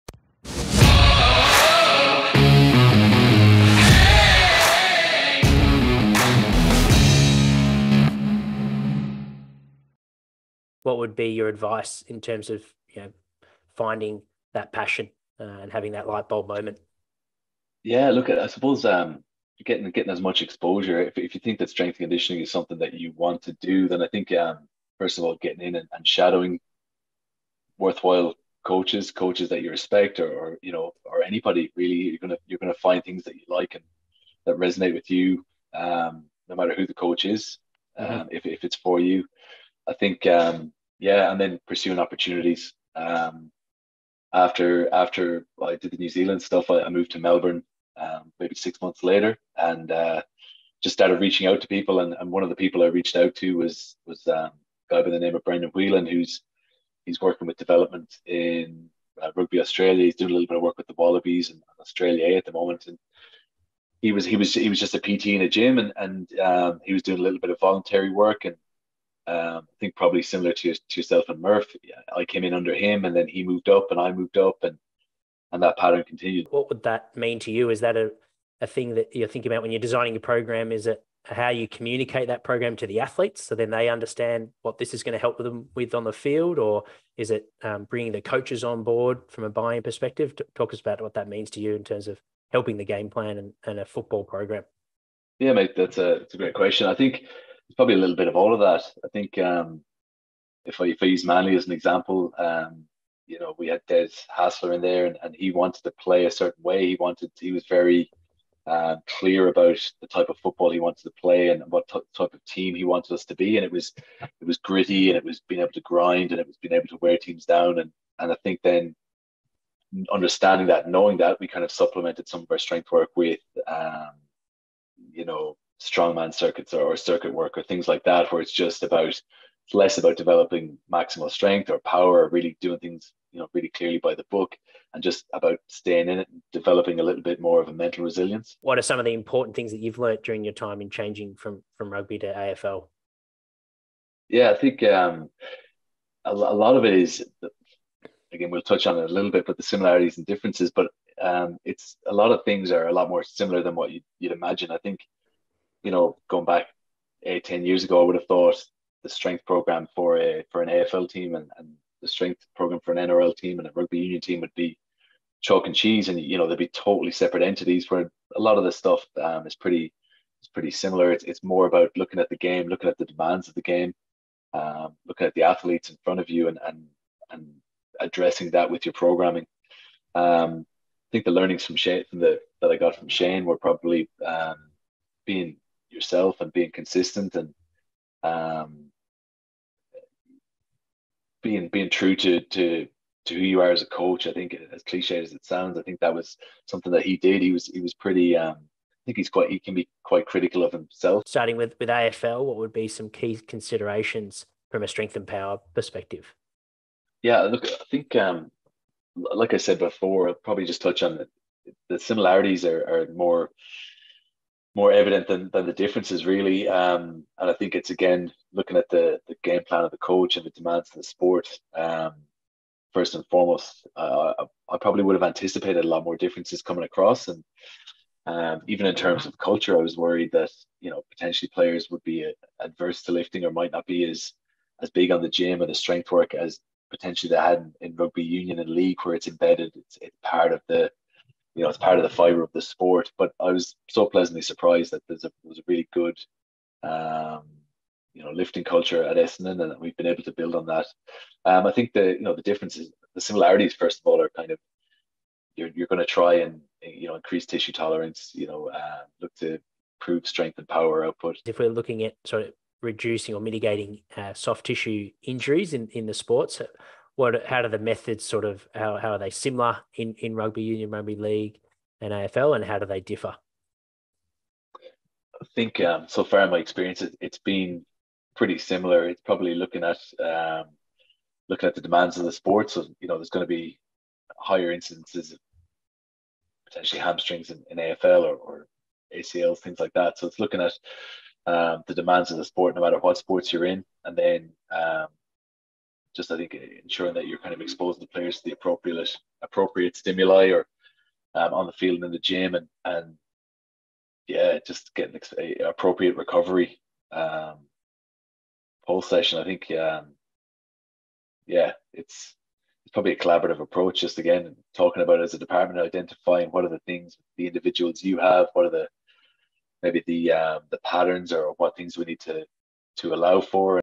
#288 - Conor Daly, Senior Strength and Conditioning Coach at Milwaukee Bucks Inc.
Prior to Milwaukee Bucks, Conor Daly was the lead Strength and power Coach at Essendon FC. He is an experienced Irish Strength and Conditioning Coach working in the sports industry and skilled in athlete development, strength coaching, speed & agility, and Olympic lifting. Conor is a strong communicator and professional coach with a Master of Science (M.Sc.) focused in Strength and Conditioning from Edith Cowan University.

Highlights from the episode:

00:00 Tips to help strength & conditioning coaches find their passion in the fitness industry
01:16 The importance of the physical preparation program assisting the football game plan
02:18 The similarities and differences between rugby union, league and AFL
03:21 The factors to consider when prescribing strength & power sessions prior to field sessions
04:21 The importance of understanding the demands of the game style and how to develop physical qualities to enhance performance and mitigate injuries

Conor Daly, Senior Strength and Conditioning Coach at Milwaukee Bucks Inc., advises aspiring strength and conditioning professionals to gain exposure by shadowing respected coaches and pursuing opportunities in the field.

Daly highlights the importance of finding elements that resonate with individuals during the shadowing process, emphasizing that passion for strength and conditioning can be discovered by working with coaches from various backgrounds.

The coach shares his own journey, mentioning how he reached out to professionals in the industry, including Brandon Wheelen in Rugby Australia, to gain experience and advance in his career.

In discussing program design for athletes, Daly emphasizes the need for a multi-faceted approach, considering the coach's game plan, effective communication of the program's benefits to athletes, and garnering support from coaches for successful implementation.

Daly reflects on his transition from rugby to AFL (Australian Football League) and notes that despite expectations of significant differences, the strength programs for the two sports share more similarities than differences. He highlights the importance of tailoring programs to the specific demands of each sport while maintaining a focus on increasing tissue tolerance and minimizing injuries through appropriate stimuli and recovery.

Have feedback on the show? We really would love to hear from send us your feedback, tell us what you love and what you think could be better. And, if you’re really enjoying the show, please leave us a review.